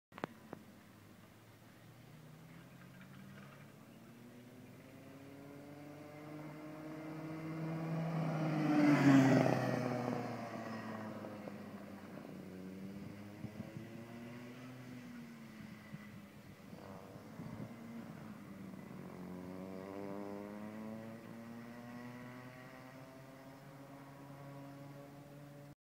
Ekaterina Luescher CRG Rotaxmax micro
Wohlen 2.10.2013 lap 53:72
Ekaterina 6years